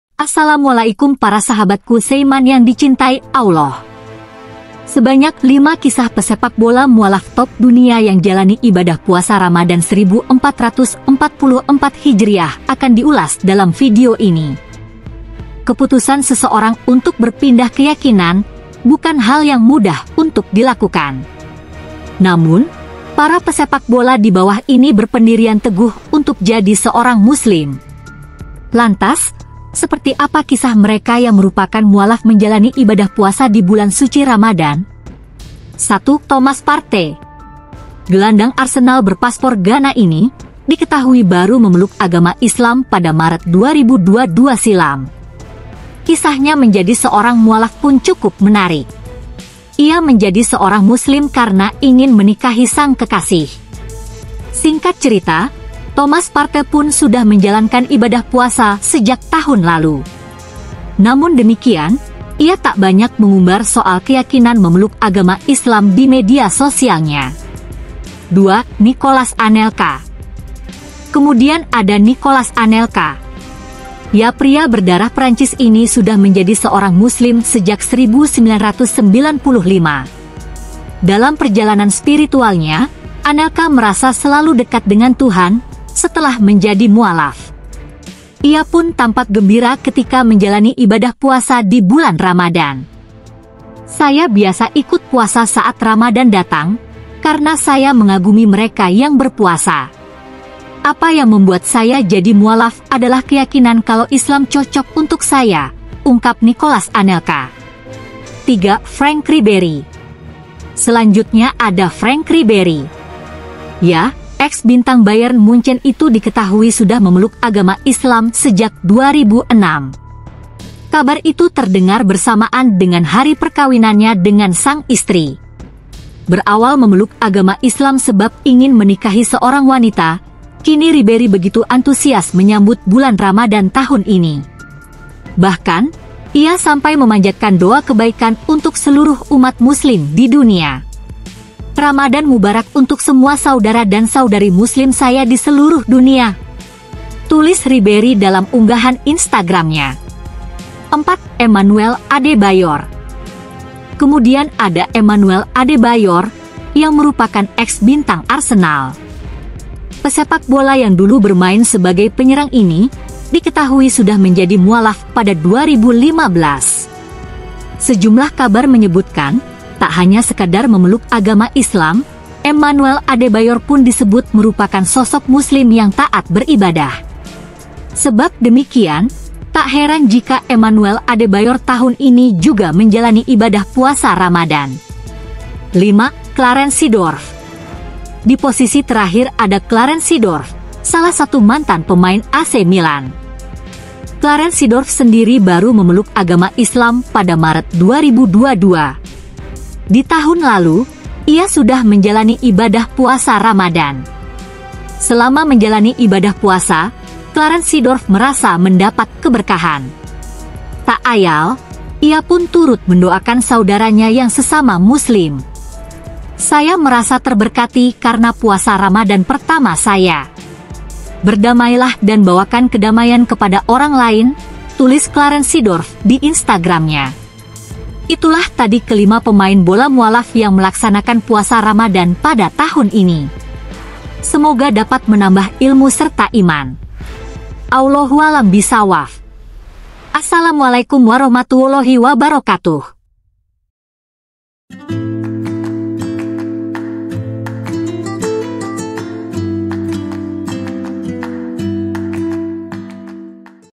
Kisah 5 Pemain Bola Mualaf yang Menjalani Puasa pada Tahun Ini, Siapa Saja?
Pada video ini menceritakan dari Pesepakbola mualaf yang menjalani puasa di tahun ini. Inilah kisahnya!